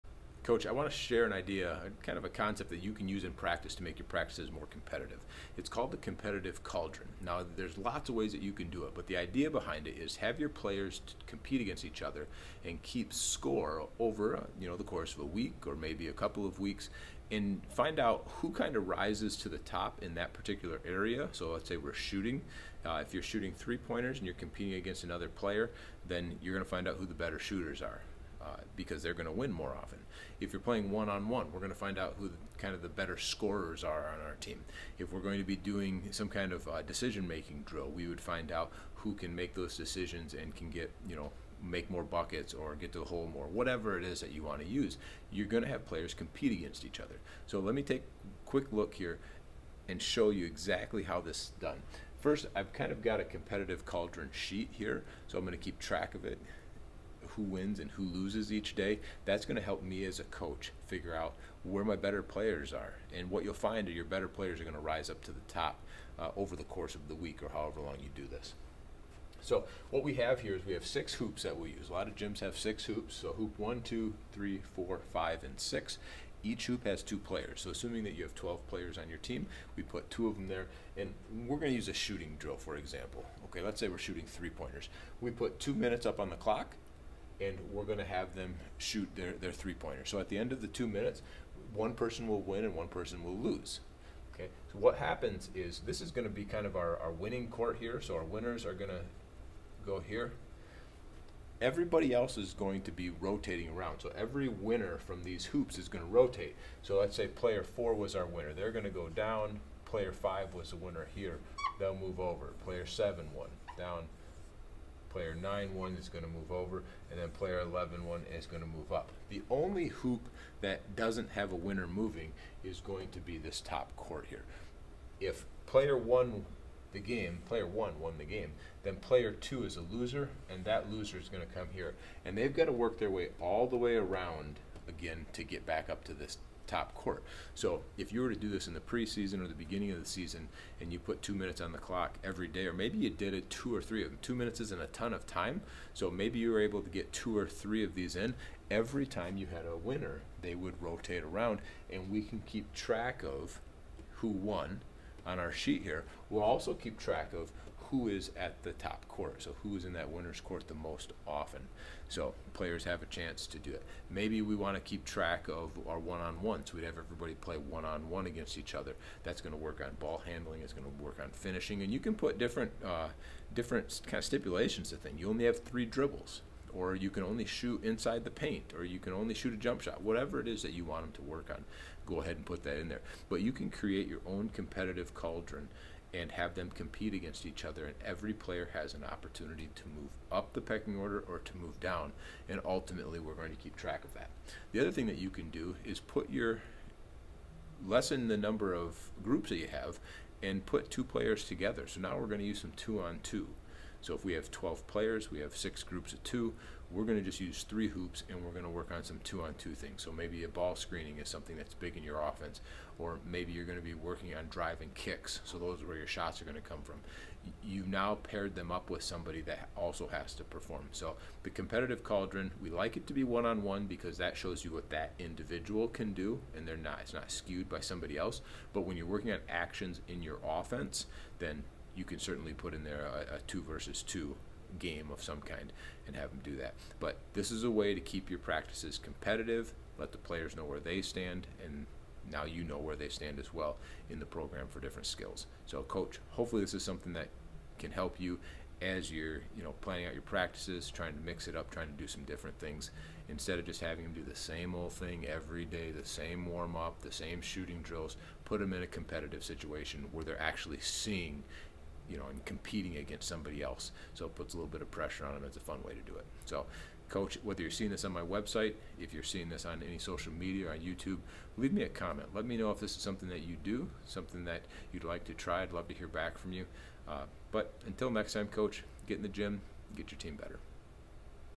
More Competitive Practices - Competitive Cauldron
Coaches talk about competitive practices but how do they do it? A competitive cauldron is one way to add intensity and competition to your practice. In this video, I share one way you can do this.

For the best basketball coaching resources join the Coaching Lab